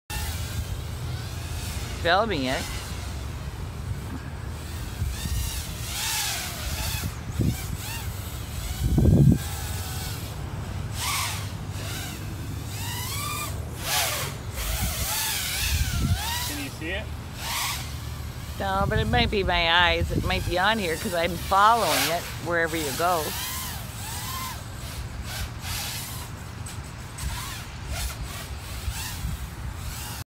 newquad
just a new build for a friend trying it out.